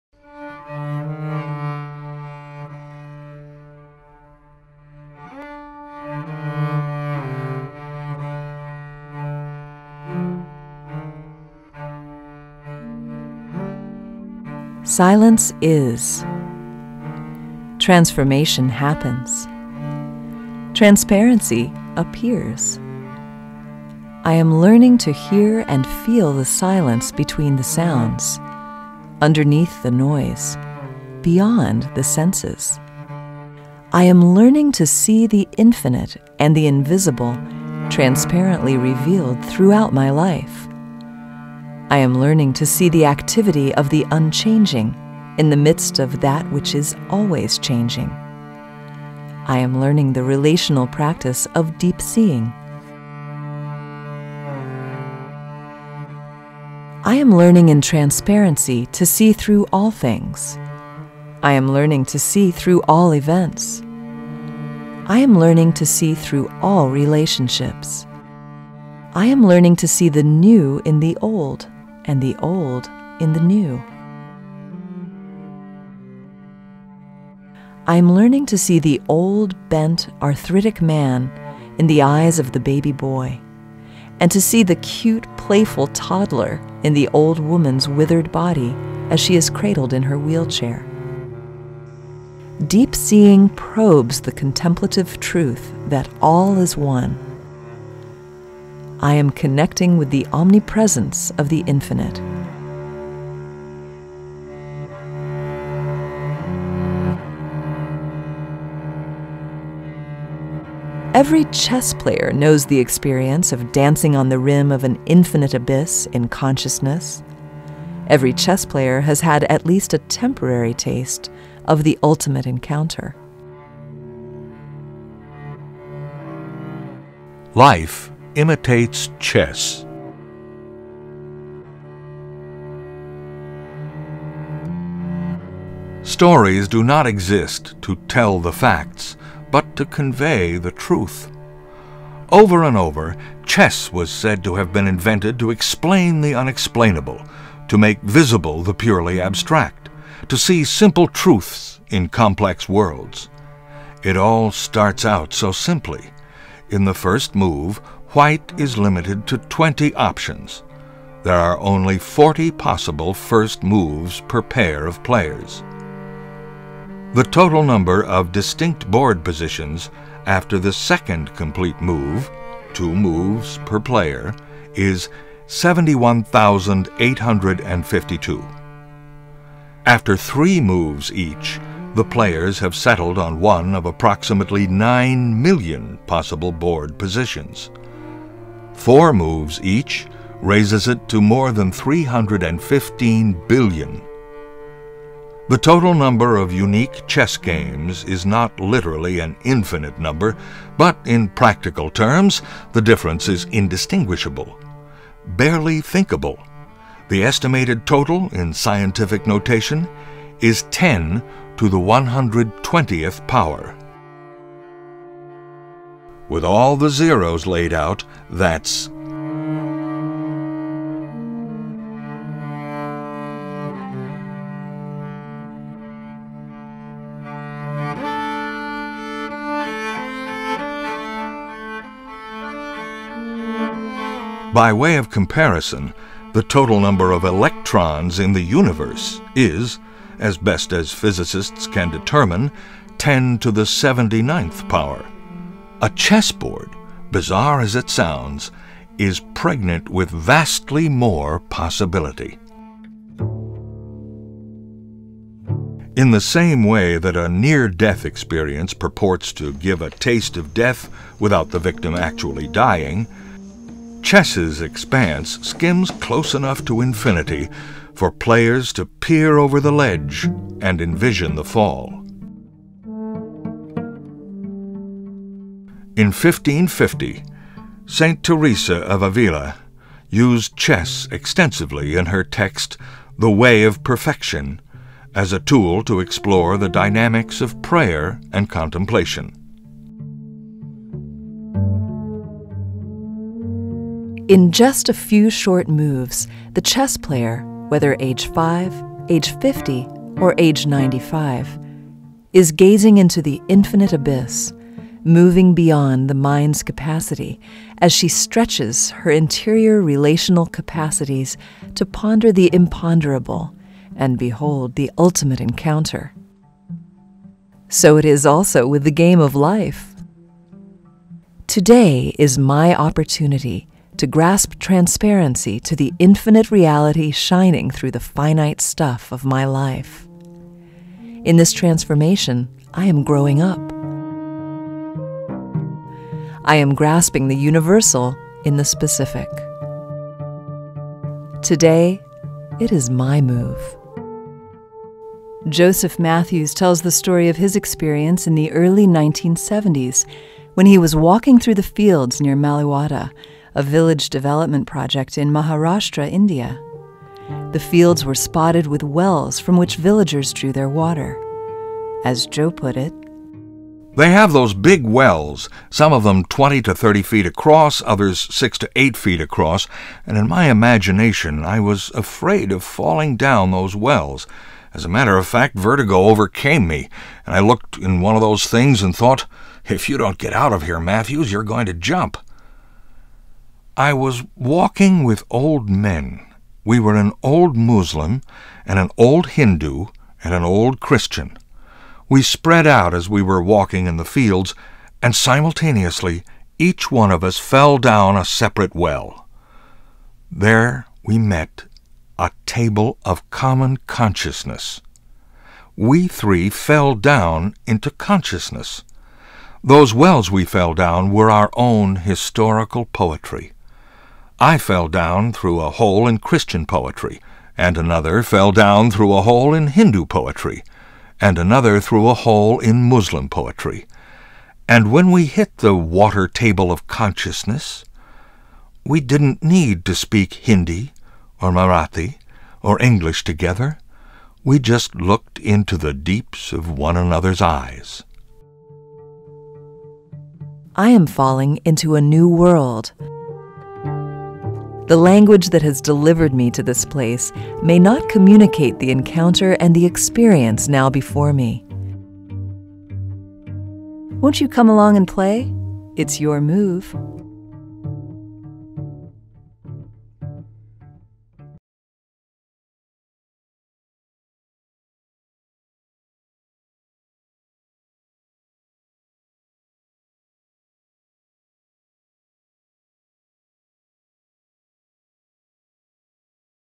The Transparency - The Relational Practice of Deep Seeing
How does one see the invisible revealed?  What is the activity of the unchanging, in the midst of that which is always changing?   Hear about the next step - Unity